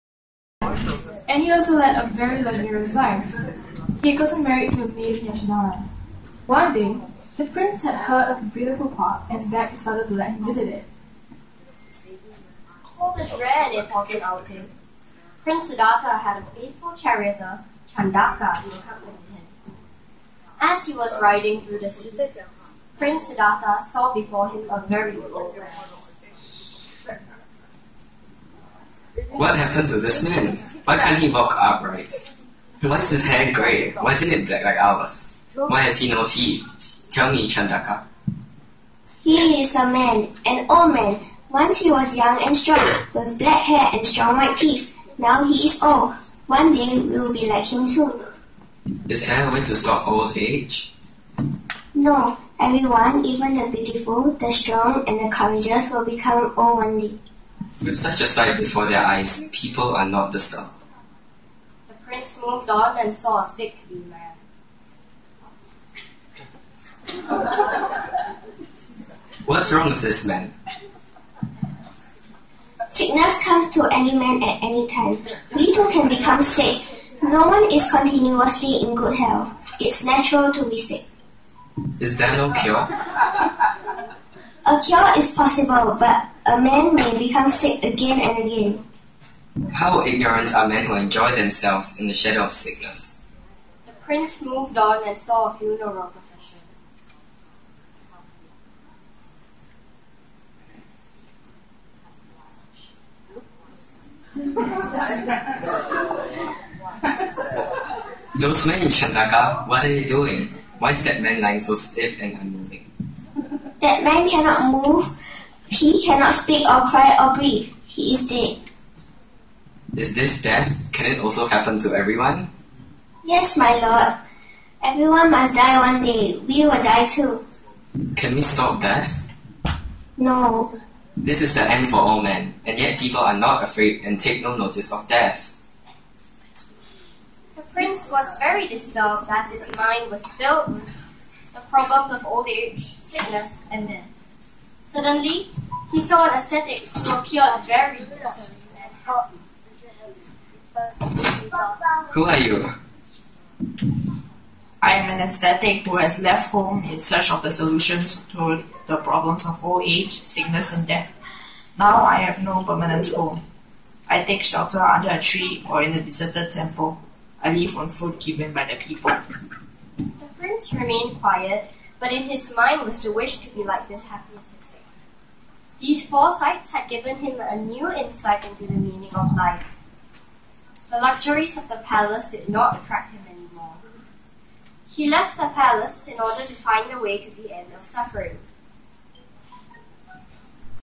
Buddhist Library Sunday School Vesak Day 2017 Play
The story of The Buddha.